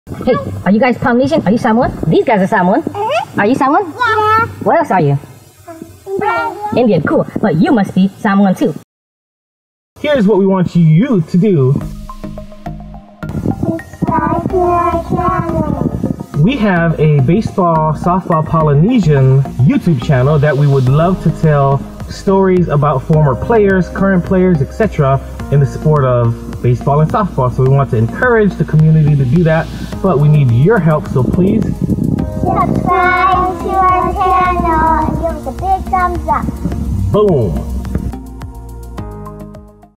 Welcome to Baseball Softball Pacific Rim
From college schoalrships to professional contracts and getting drafted by professional teams, there are a plethora of amazing talent and our channel will be the one to share their stories with you!!!